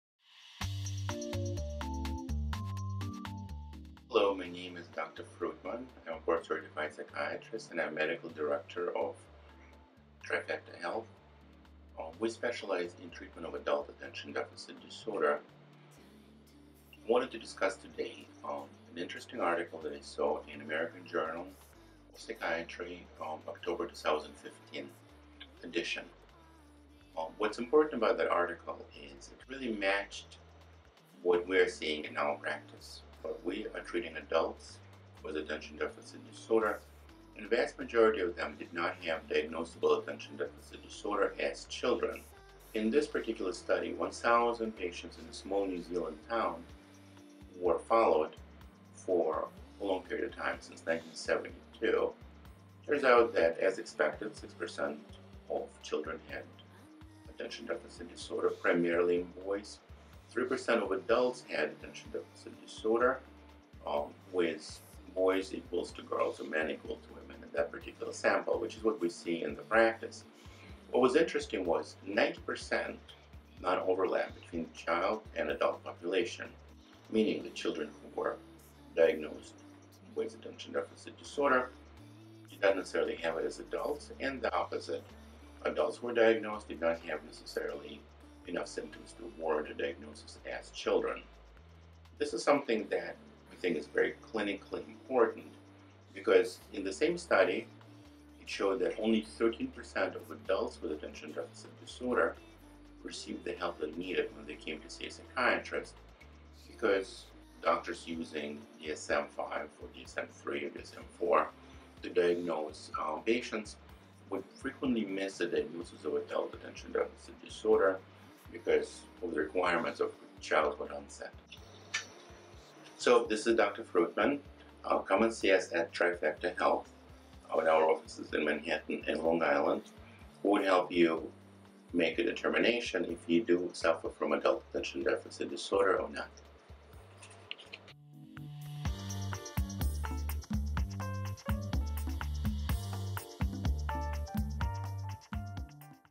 Adult vs Child ADHD : What Is The Difference?
Is Adult ADHD a Different Disorder Than Childhood ADHD?

Many adults may suffer from ADD/ ADHD and not know it. Like ADHD in children and teens, ADHD in adults can make life challenging. NYC Psychiatrist Dr. Edward Fruitman, M.D. explains the difference between Adult and Child ADHD.

Trifecta Health is dedicated to helping professionals working in Manhattan area of New York City to get rapid relief of their ADD/ ADHD symptoms.